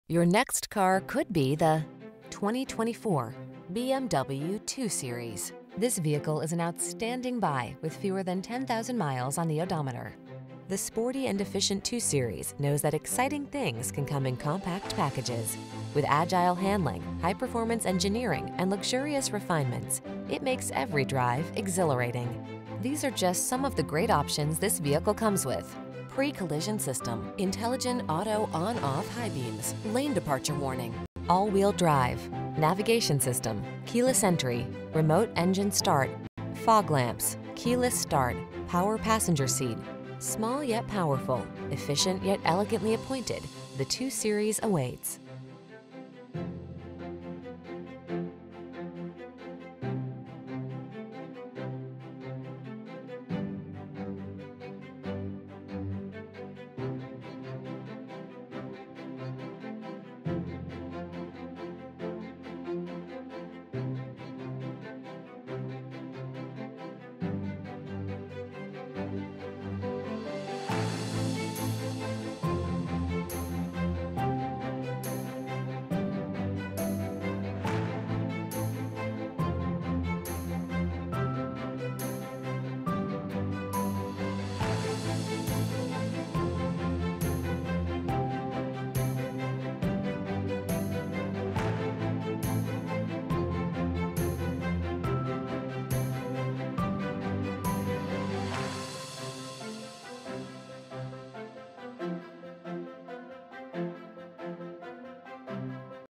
2024 BMW 2_Series 228i xDrive IL Chicago, Evanston, Highland Park, Deerfield, Northbrook
Pre-Owned 2024 BMW 2_Series 228i xDrive available at Fields BMW Northfield located in 700 Frontage Rd, Northfield, IL, 60093. Servicing the Chicago, Evanston, Highland Park, Deerfield, Northbrook area.

Your next car could be the  2024  BMW 2_Series.  The sporty and efficient 2 Series knows that exciting things can come in compact packages. With agile handling, high-performance engineering, and luxurious refinements, it makes every drive exhilarating. These are just some of the great options this vehicle comes with: Pre-Collision System, Intelligent Auto on/off High Beams, Lane Departure Warning, All Wheel Drive, Navigation System, Keyless Entry, Remote Engine Start, Fog Lamps, Keyless Start, Power Passenger Seat. Small yet powerful, efficient yet elegantly appointed. The 2 Series awaits.